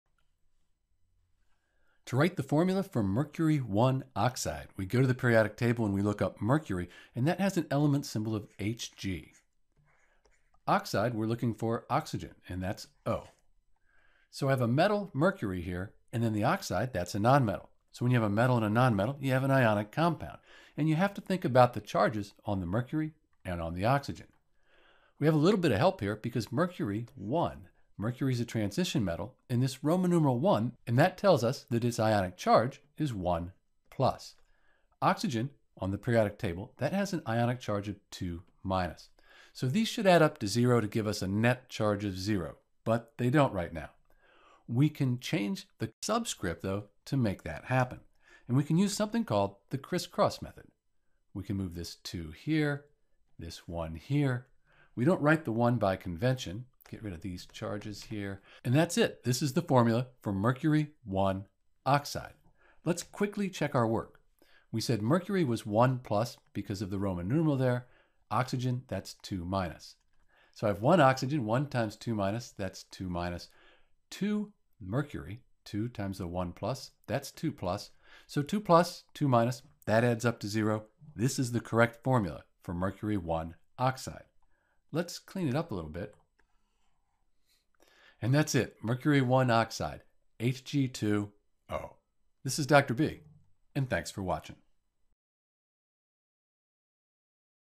How to Write the Formula for Mercury (I) oxide
In this video we'll write the correct formula for Mercury (I) oxide, Hg2O.

To write the formula for Mercury (I) oxide we’ll use the Periodic Table and follow some simple rules.

1. Write the symbol and charge for the transition metal. The charge is the Roman Numeral in parentheses.
2. Write the symbol and charge for the non-metal. If you have a polyatomic ion, use the Common Ion table to find the formula and charge.
3. See if the charges are balanced (if they are you’re done!)
4. Add subscripts (if necessary) so the charge for the entire compound is zero.
5. Use the crisscross method to check your work.

Notes:
For a complete tutorial on naming and formula writing for compounds, like Mercury (I) oxide and more, visit: